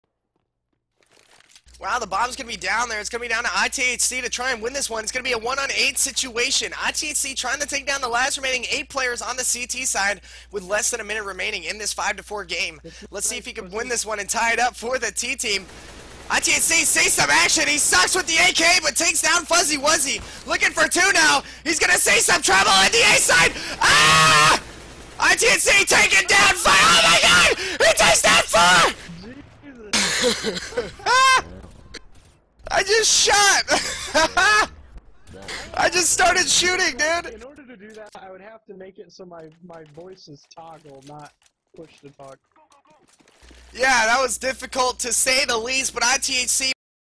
Quad Kill While Shoutcasting - eyeTHC
Just doing my normal shoutcasting in casual and managed to get this clip!

to join us on Teamspeak and our hosted servers for various games!